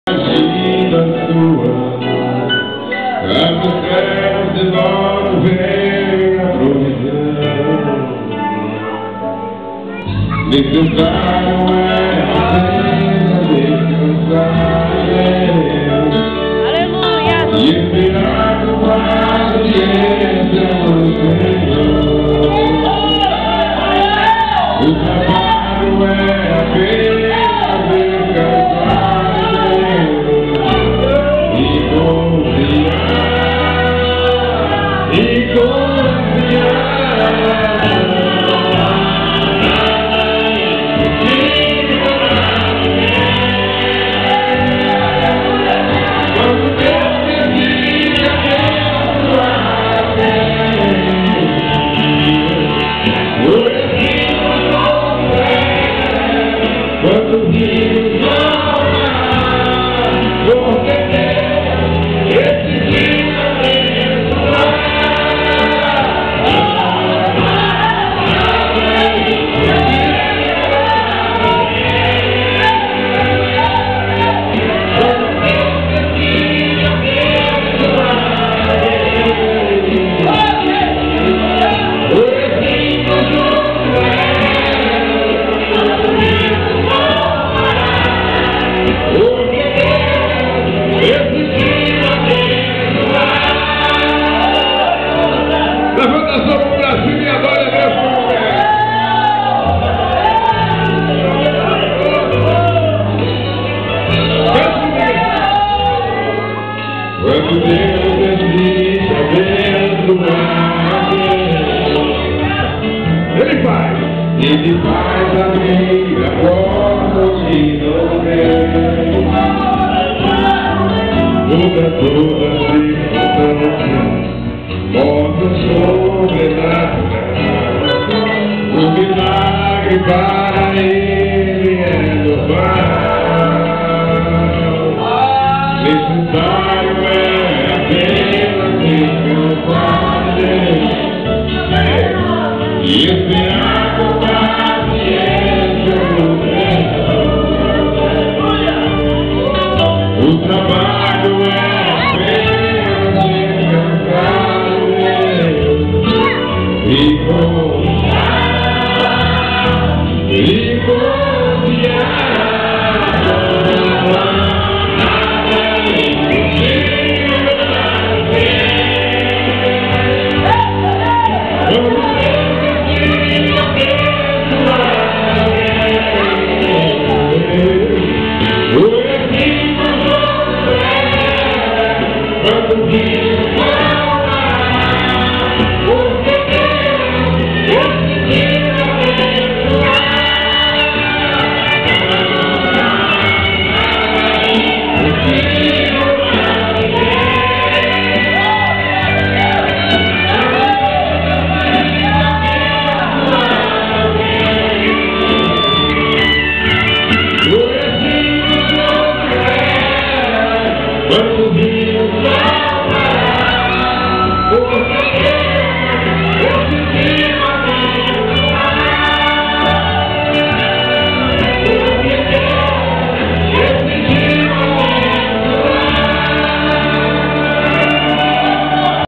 SEBASTIÃO BEZERRA CANTANDO NA AD CIADSETA EM NOVA CRIXÁS GO
CULTO DOS VARÕES